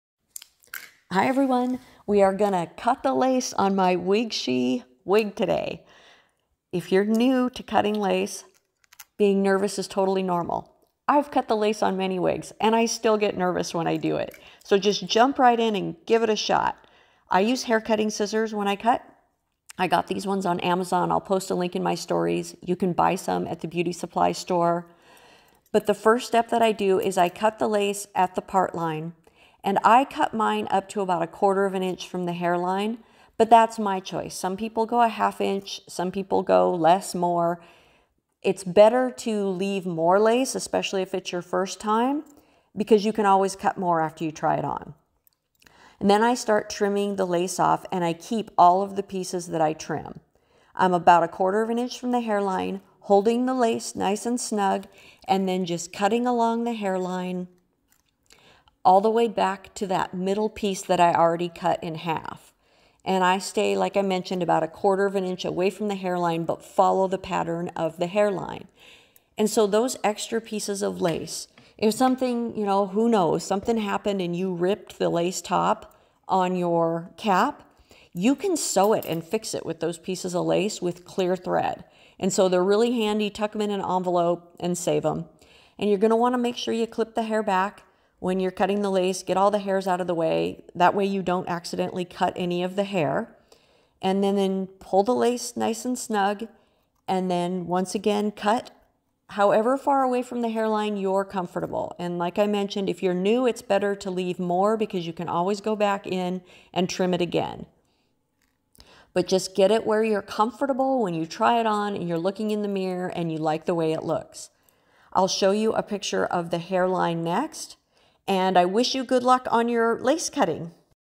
Let's Cut The Lace On A Human Hair Wig!
Let's cut the lace on my @wigshehair!

If cutting lace makes you nervous, you are not alone. The first time I had to cut the lace I was extremely nervous. I still get nervous!😬
So let's be nervous together!

Tips:
▶️Get a glass of wine and drink it🍷😄kidding! But if it calms your nerves who am I to say No?🤷‍♀️

▶️Use very sharp scissors so you get a clean edge on the lace while cutting.

▶️Some people prefer to cut lace with pinking shears. This method leaves a scalloped edge. I haven't personally tried it.

▶️I cut to about a 1/4" from the hairline. Many do a 1/2". It's personal preference on how you want to wear your lace. Less is more when starting!
Cut the first time, try it on and see if you want to cut more.

▶️Follow the existing hairline on the wig as you cut.

▶️Save the lace you cut off. You can use it later in case you need to make a lace repair on the lace top.

Now your lace is cut! Enjoy your wig and know you just completed a big step in human hair wig wearing🥂

Wig Details:
@wigshehair WIG009

Scissors:
Amazon